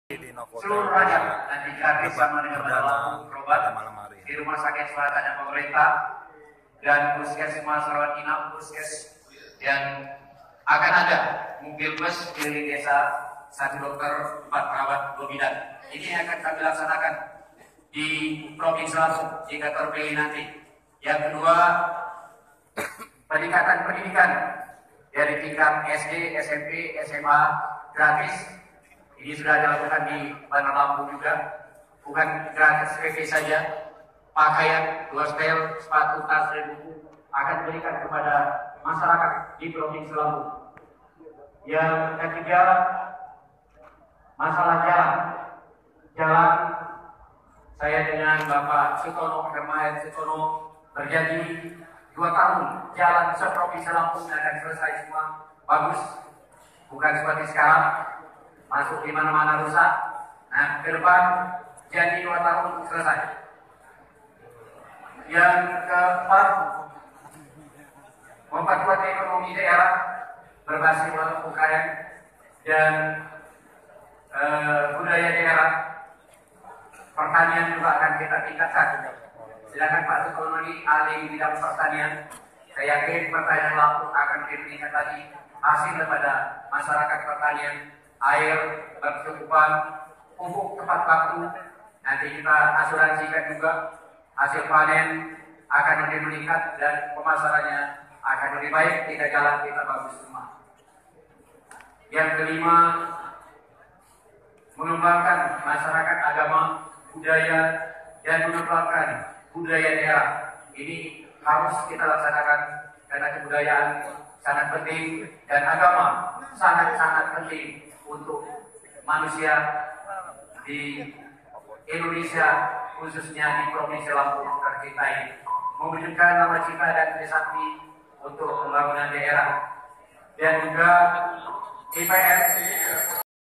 Debat Publik Paslon Pilgub Lampung 2018
Debat Publik Pilgub Lampung 2018 di Ballroom Hotel Novotel Bandarlampung, 7 April 2018.

Video Creator : Ahmad Muslimin